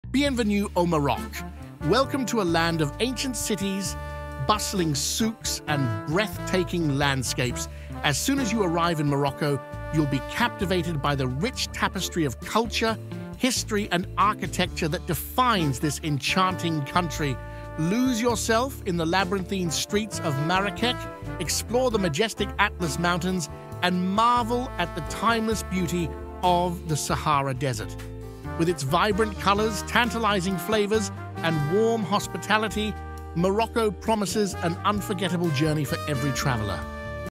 You must visit Morocco, you gonna love it!
Take a virtual journey to Morocco, where ancient cities, bustling souks, and breathtaking landscapes await. Experience the rich tapestry of culture, history, and architecture that defines this enchanting country. Let's explore together!

Photo by George John from Pexels Photo by MCK from Pexels Videos by SwissHumanity Stories from Pexels Photo by Nirav Shah from Pexels Photo by Balazs Simon from Pexels Photo by Alexandra Karnasopoulos from Pexels